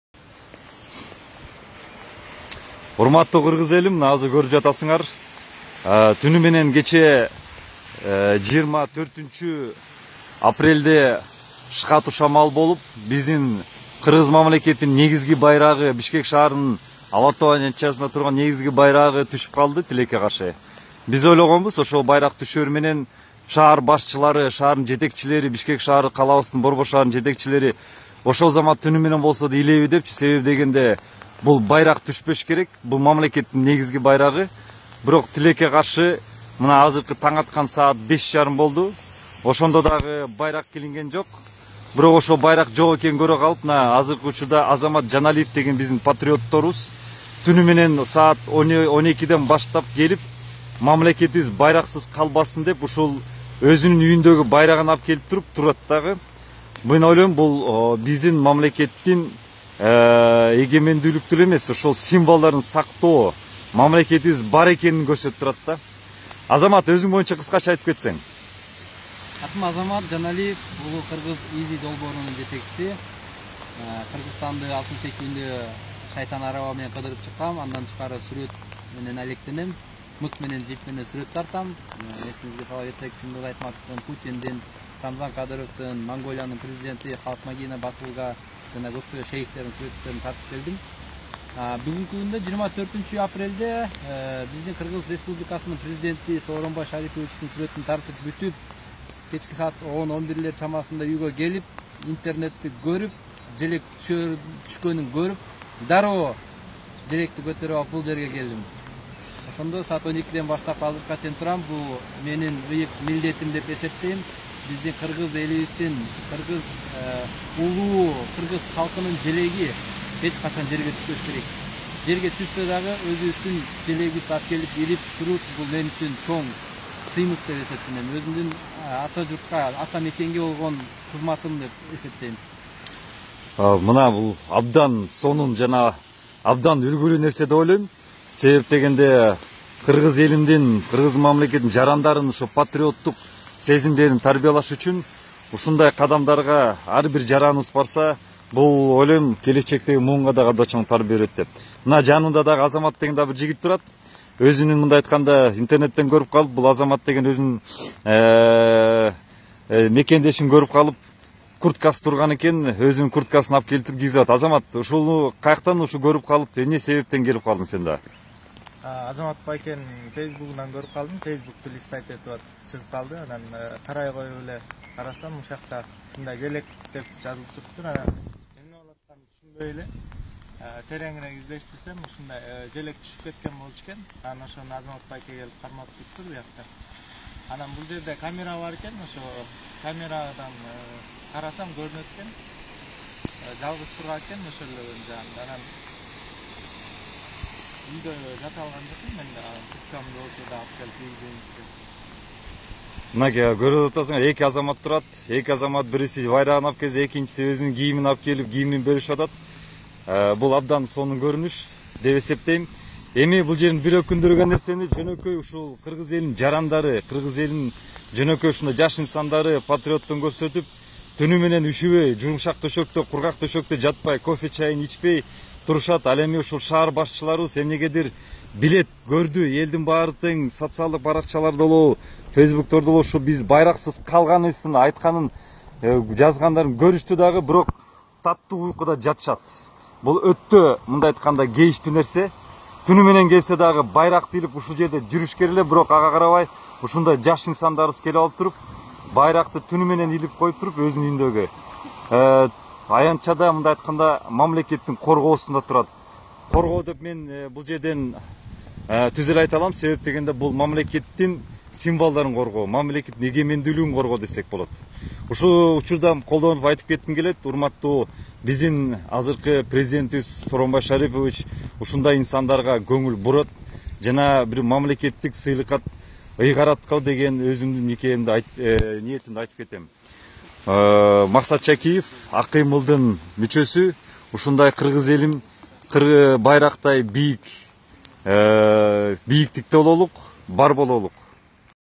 Парень простоял всю ночь с флагом на площади "Ала-Тоо", так как он упал ночью из-за сильного ветра
Видео Максата Чакиева